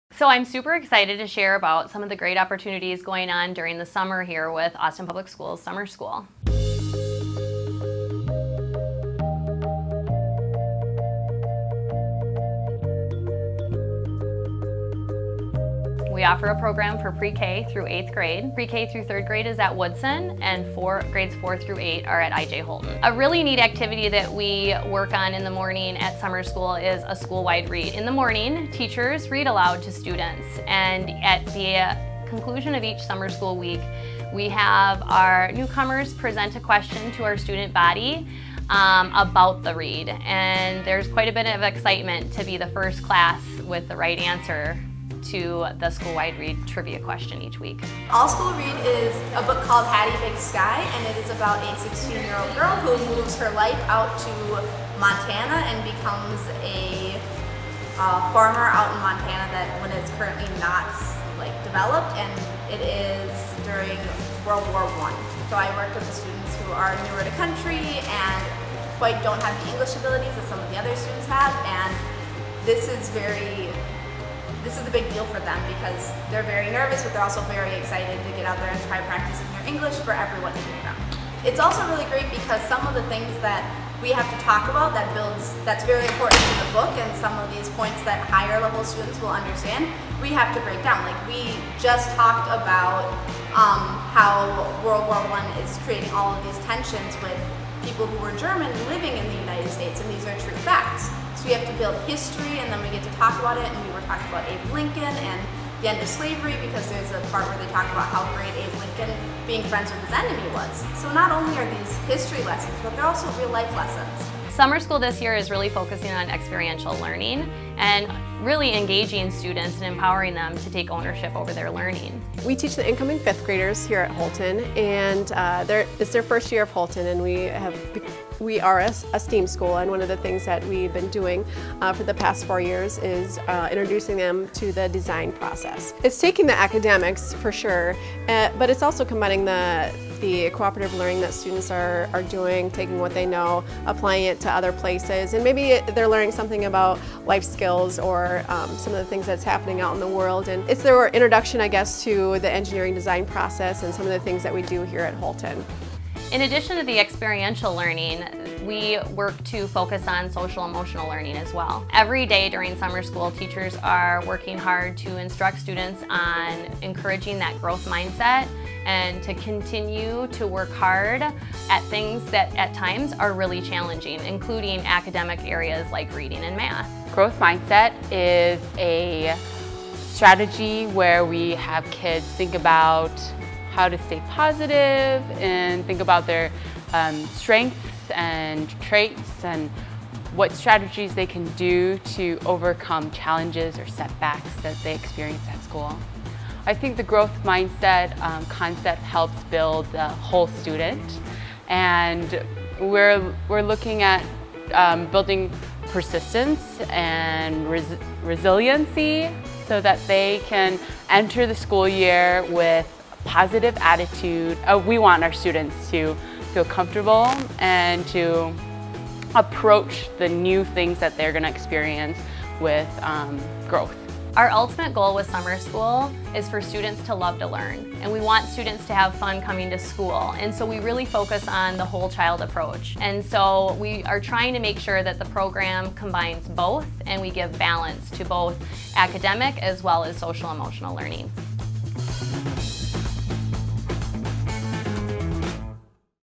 Summer School at IJ Holton
This year, Summer School for grades 4-8 is focusing on developing a growth mindset and hands-on learning.  Check out this awesome video with Summer School coordinator Jill Rollie.